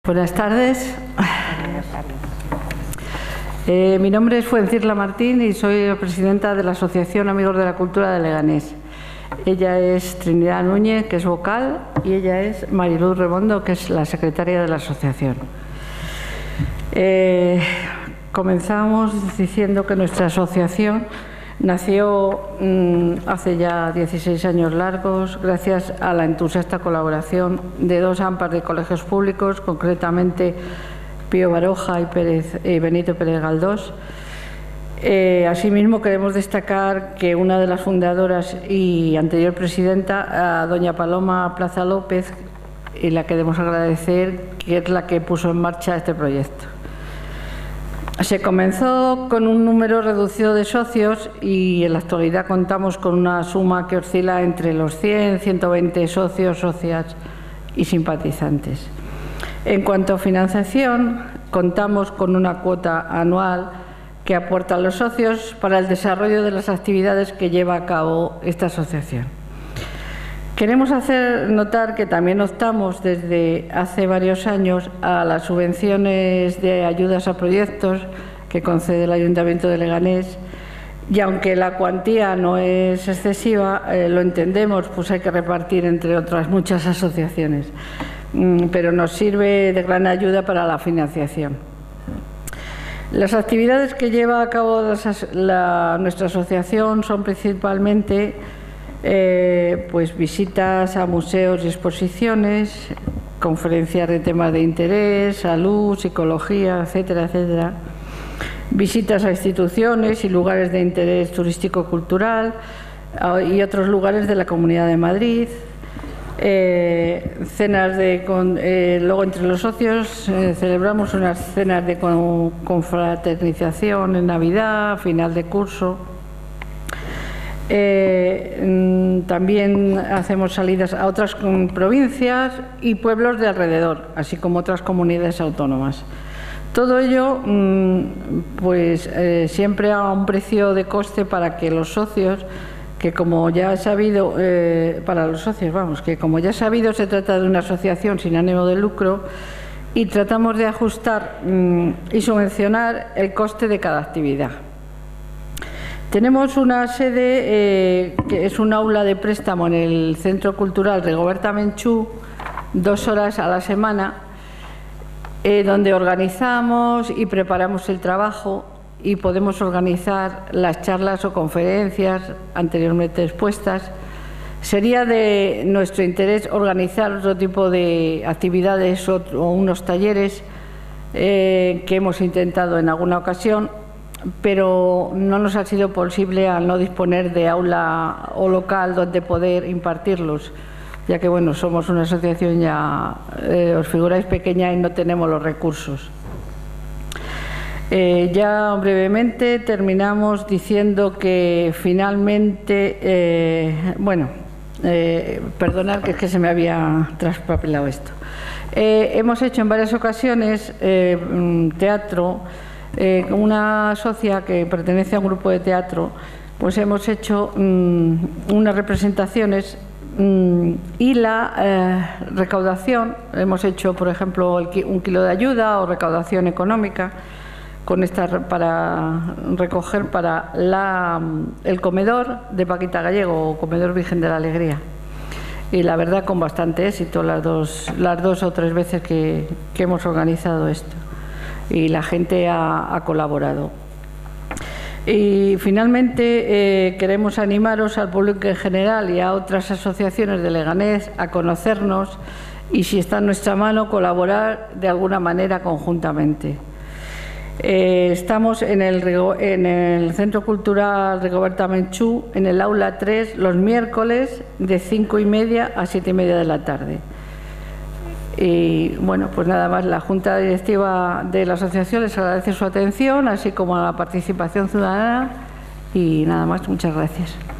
'Amigos de la Cultura' en las I Jornadas de Presentación de Entidades de Leganés
Vídeo de la presentación de la entidad 'Amigos de la Cultura' que se celebró en el marco de las Primeras Jornadas de Presentación de Entidades de Leganés, que se celebraron los días 11 y 12 de diciembre de 2012 en el teatro José Monleón del Centro Cívico José Saramago.